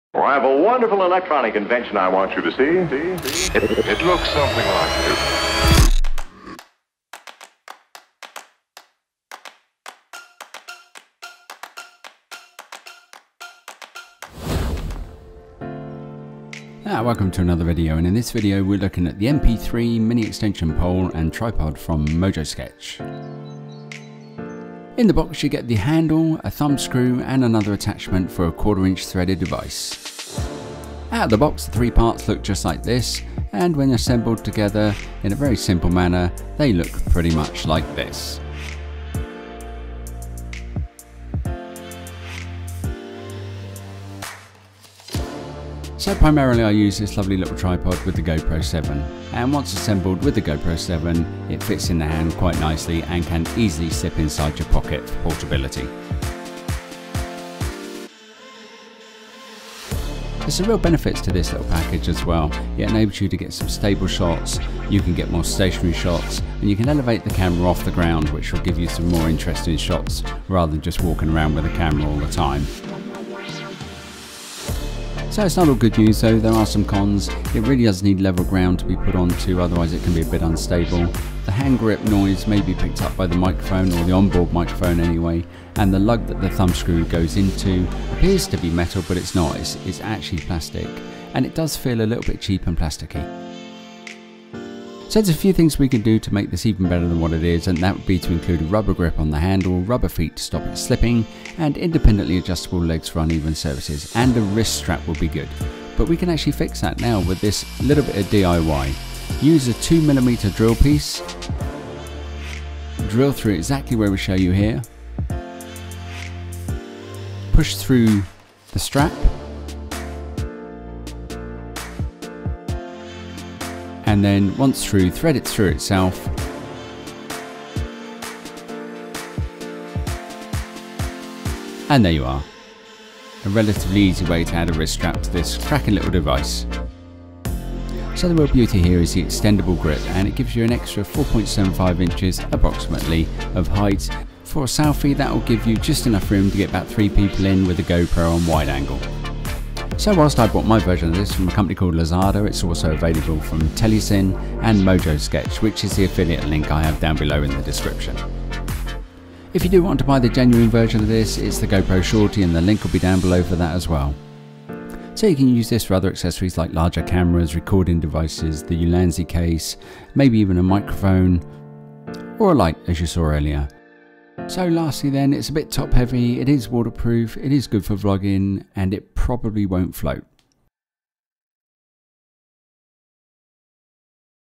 Mini Extension Pole Tripod | Mojosktch | Telesin | Lazada | GoPro Shorty
Mini Extension Poles and Tripods come in many forms. There is the Genuine GoPro Shorty and a few other versions from Mojosktch, Telesin and Lazada of the Mini Extension Pole Tripod.

Currently the availability is from Amazon for the Mojosktch mp-3 tripod: